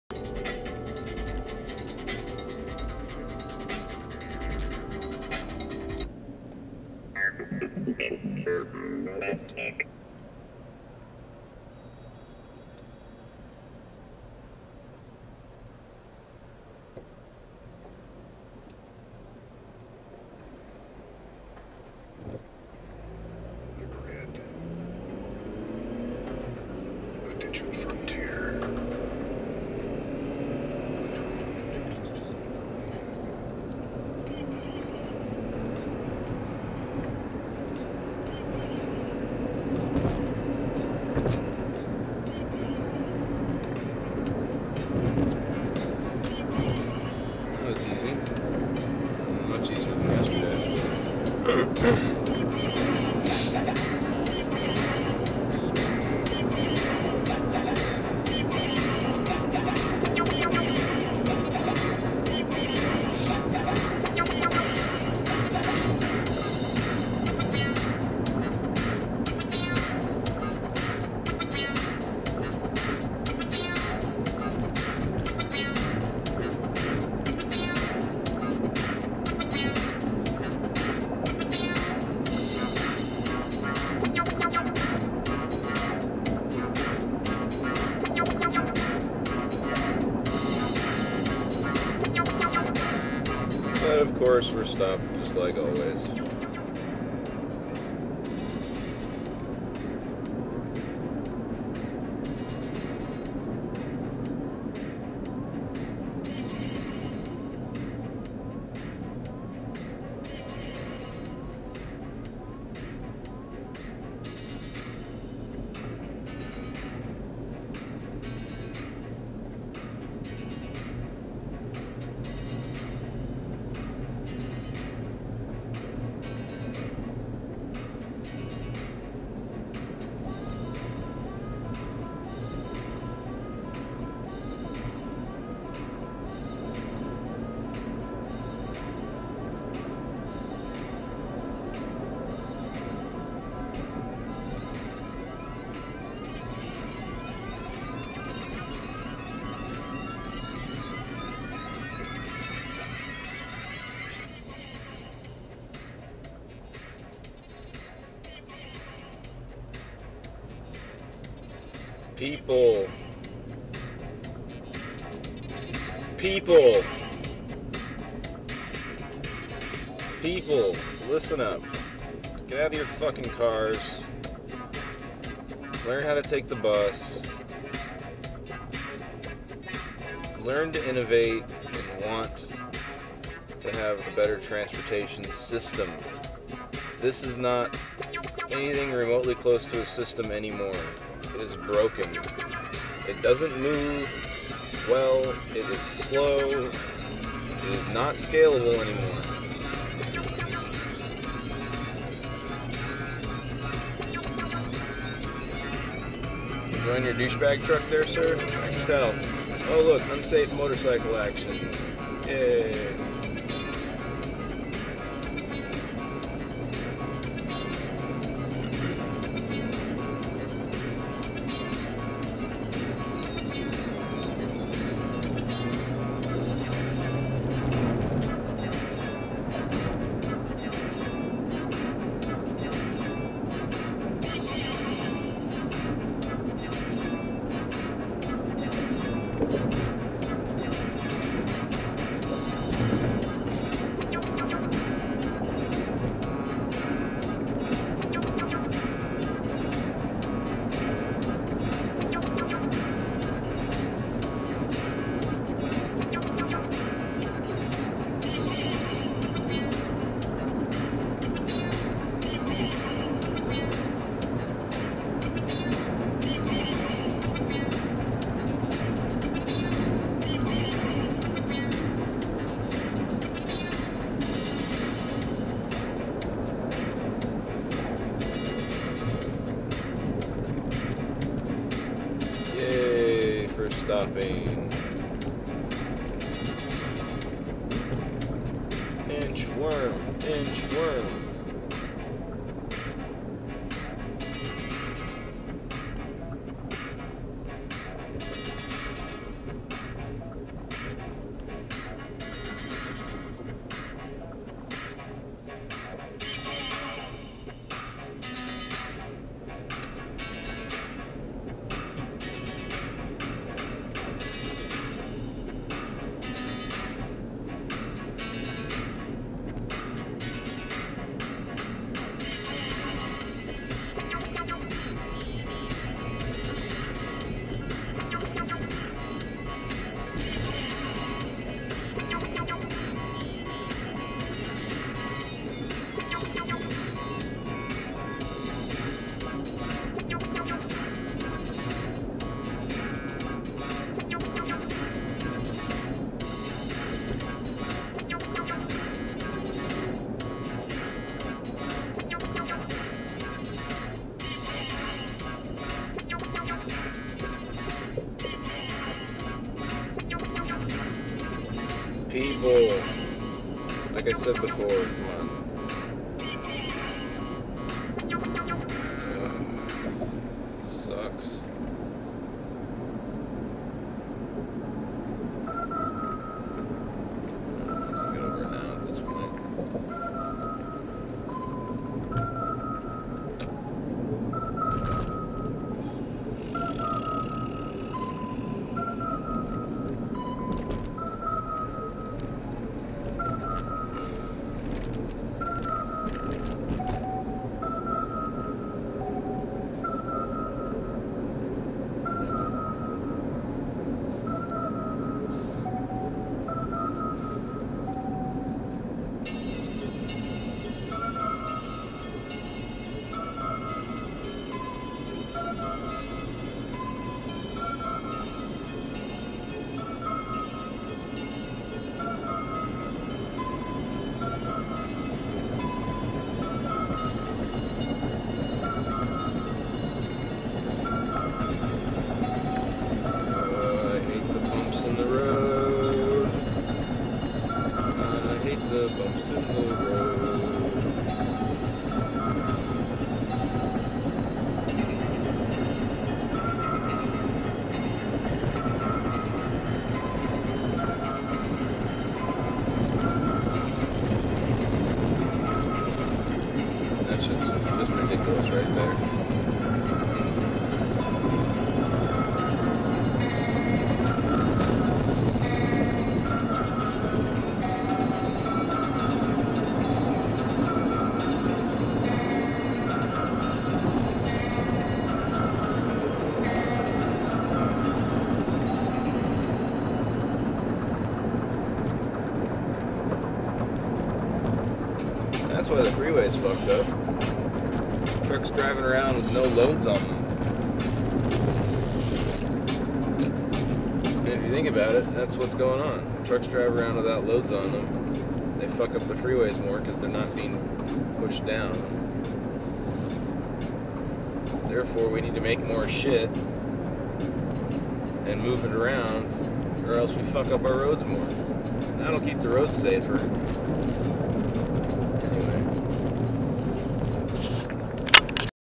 2012.02.16 Daily Driving: Saddleback, Home [2/6]
Had a good rant about drivers in the first segment. Driving around now with MORE commentary! Sometimes even special Guests. Just an example video from my Contour HD camera to show off the video quality to friends. You might want to turn down your speakers, the microphone is lacking.